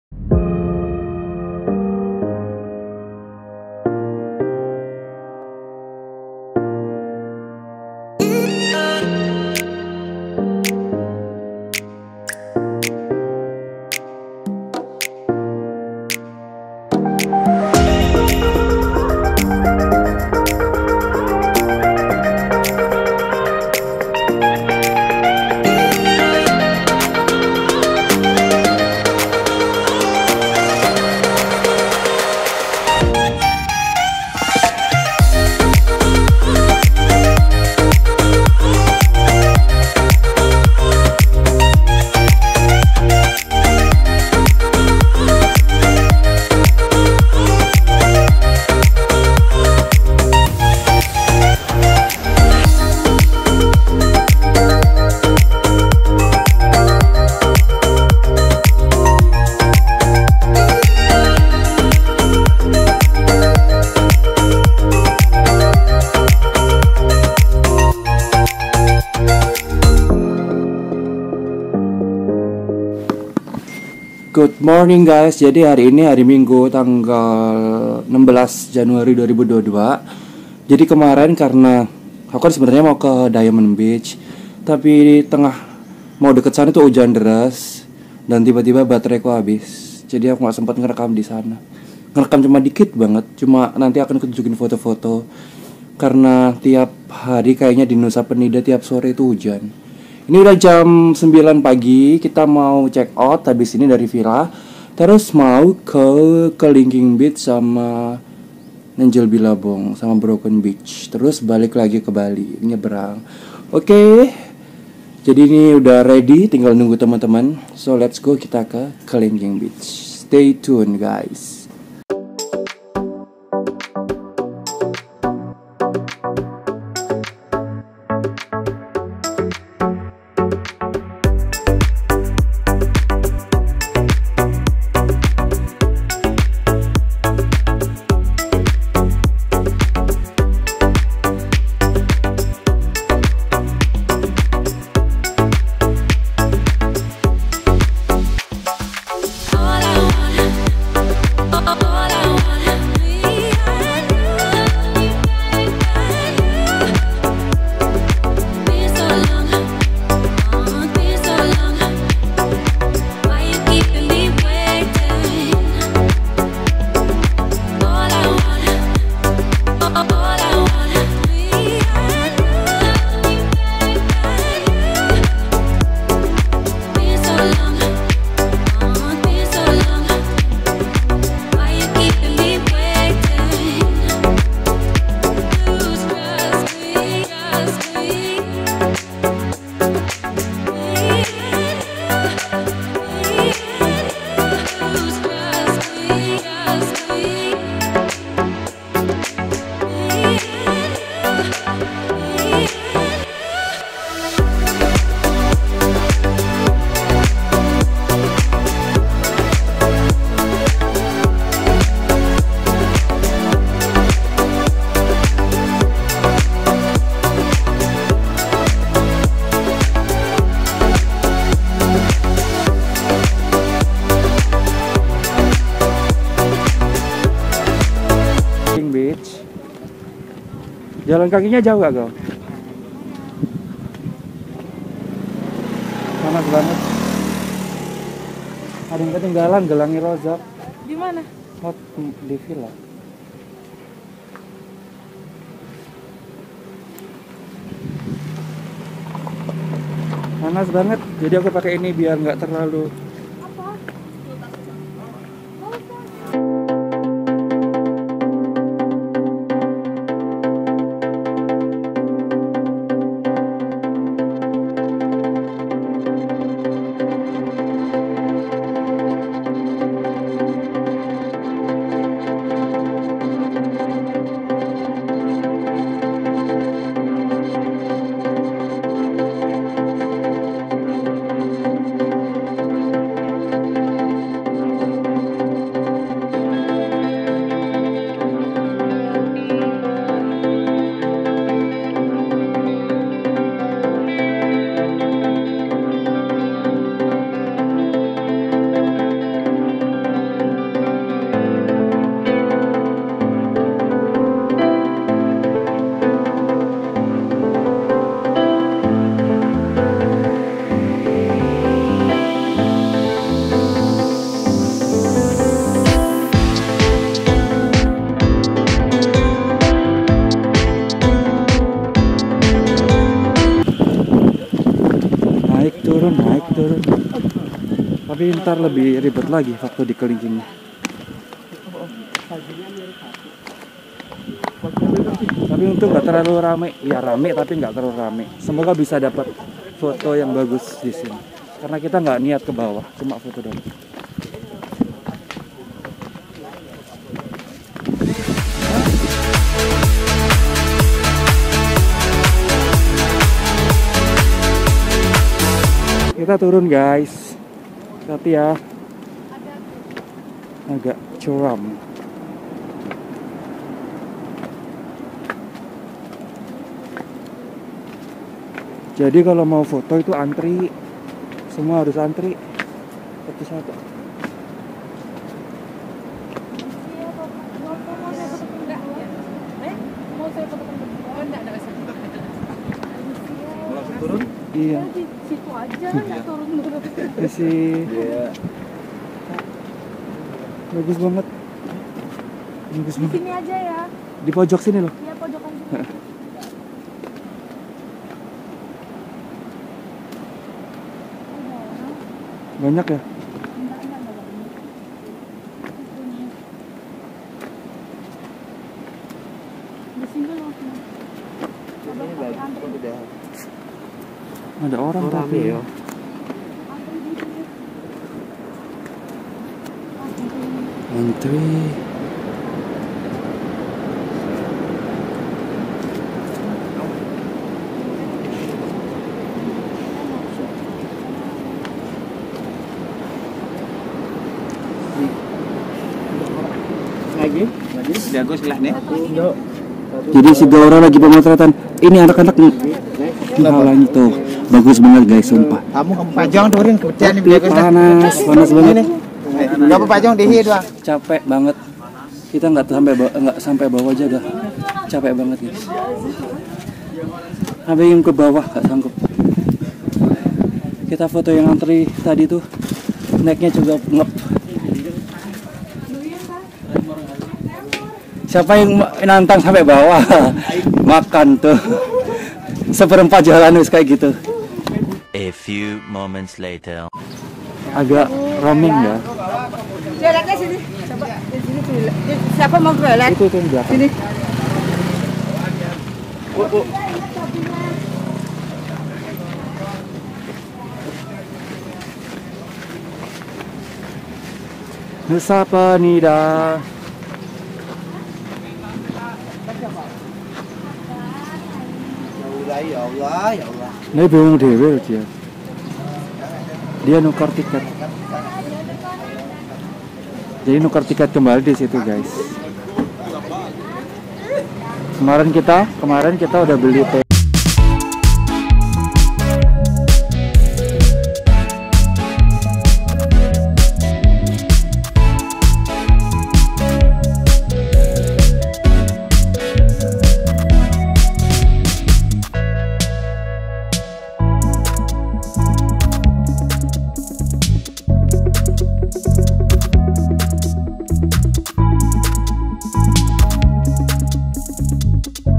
VILLA MURAH DI NUSA PENIDA & KELINGKING BEACH
Halo, kali ini Adhet Saputra kembali jalan-jalan ke Bali, ke Nusa penida lebih tepatnya. Yuk cari tau bagaimana sih paduan menemukan Villa murah di Nusa Penida saat pandemi seperti ini? Kita juga bakal sedikit explore perjalanan ke Kelingking Beach!. Langsung tonton ya!
Biaya:
Harga Villa Nusa Penida: Rp. 150.000,-/kamar
Tiket Parkir Kelingking Beach: Rp. 10.000,-/motor